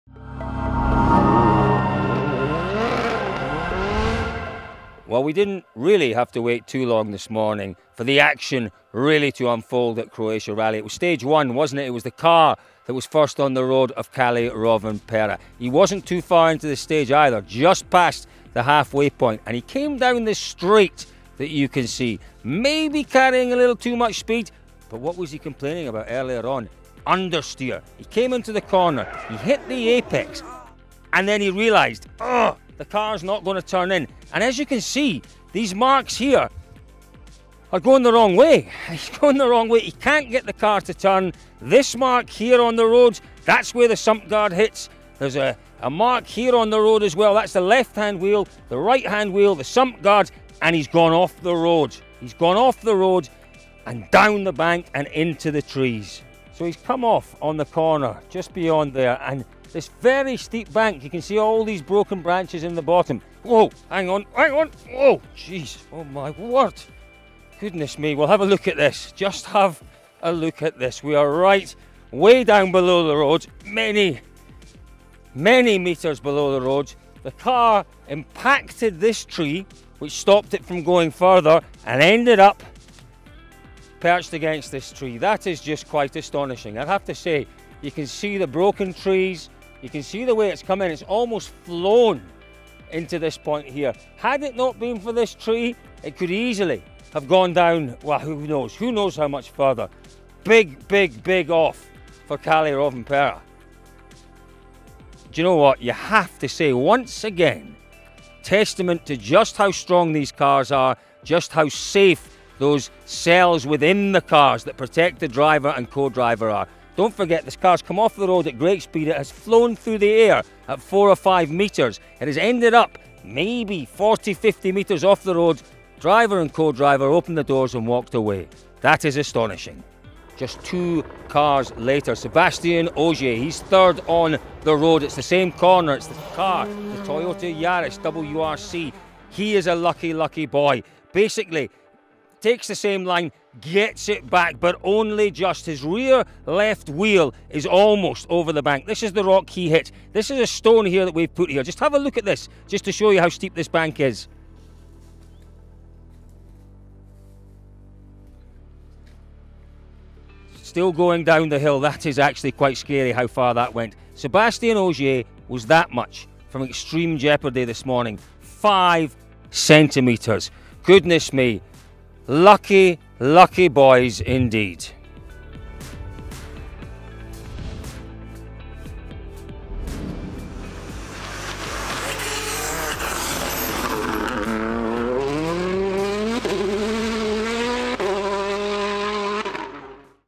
Rovanperä's Terrifying Crash Explained - WRC Croatia Rally 2021
Following Kalle Rovanperä's crash, it took us quite some time to work out where the car ended up and terrifyingly how it got there. We talk about what went wrong and explain it all to you to understand the severity of the situation.